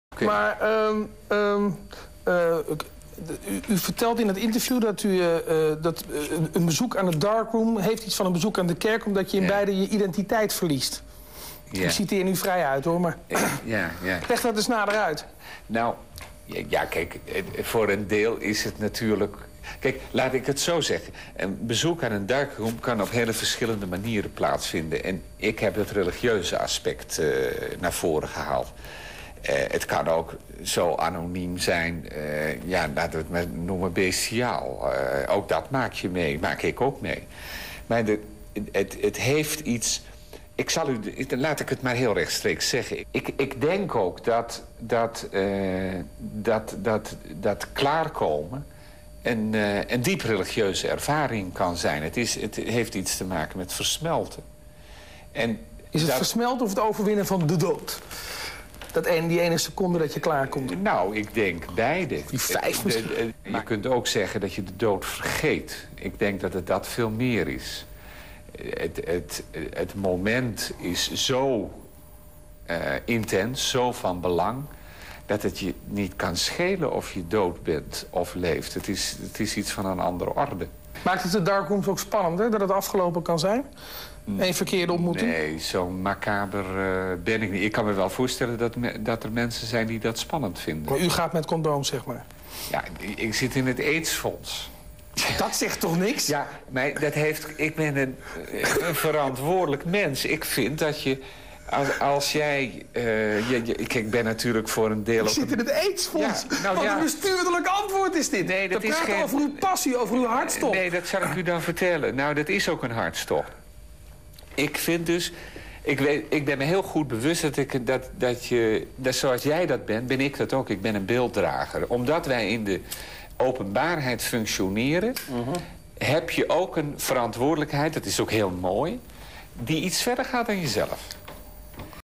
Pim Fortuyn over darkroombezoek
Pim Fortuyn vertelt over darkroom bezoek als religieuze aangelegenheid tegenover Theo van Gogh